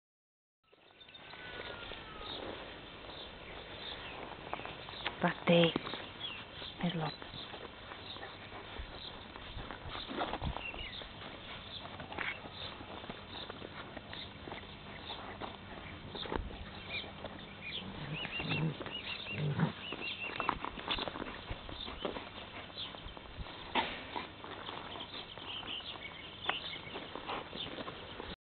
komondor puppies from Fehér Kócos with little cat
our komondors loves animals living with us